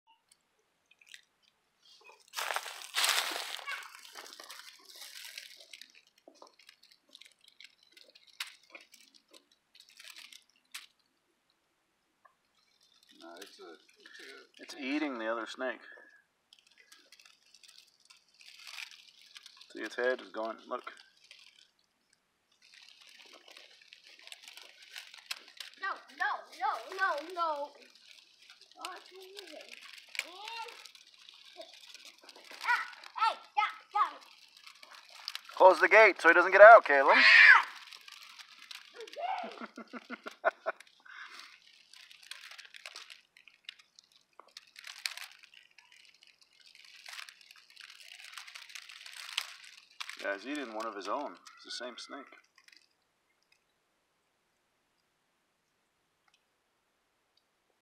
Snake Eats Snake part 1
Female King Snake Eats Male King Snake After Breeding in our backyard